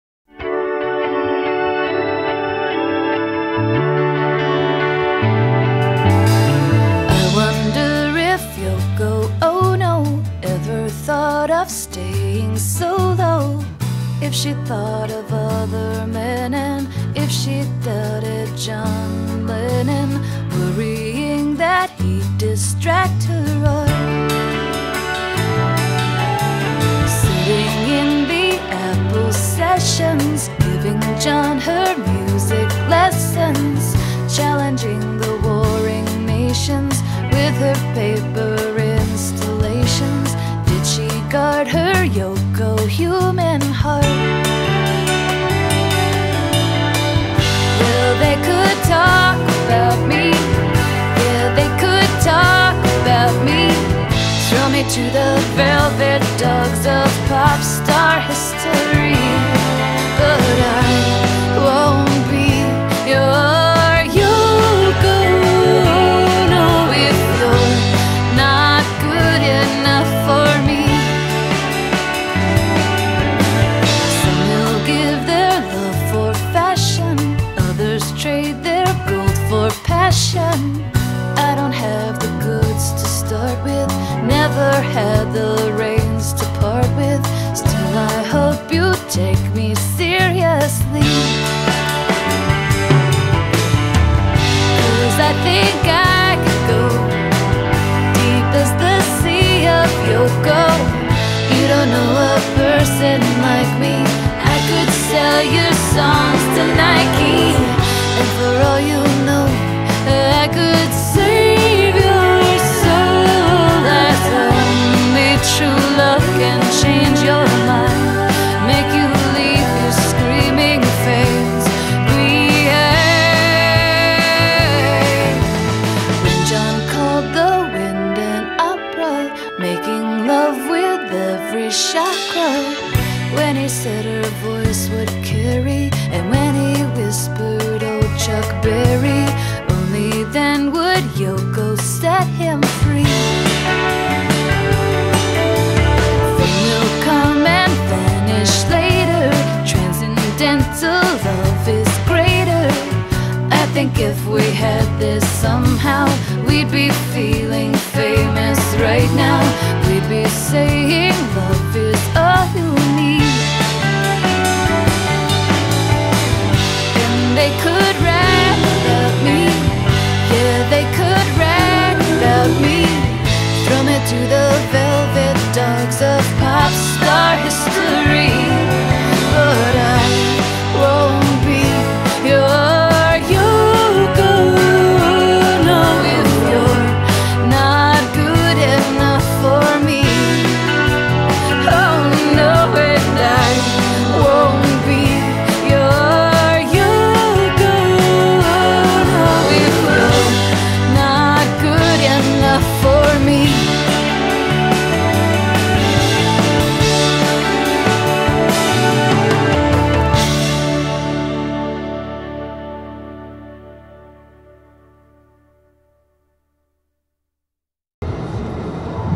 Country Club II / Avondale Neighborhood Driving Tour - Amarillo, TX
Driving through Country Club II / Avondale neighborhood in Amarillo, TX.